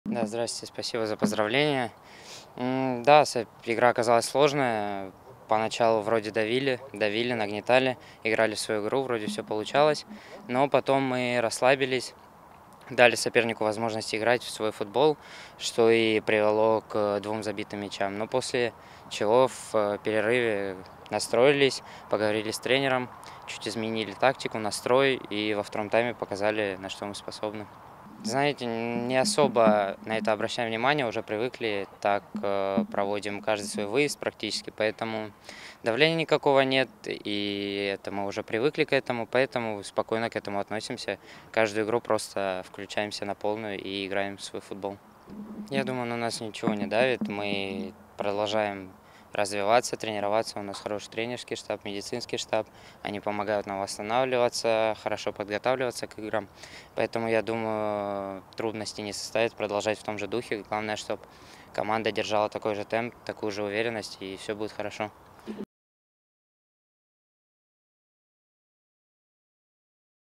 Артём Хмарин: «Главное, чтобы команда поддерживала такой темп»
🎤 Комментарий защитника «быков» после матча СШОР «Зенит»-U18 – «Краснодар»-U18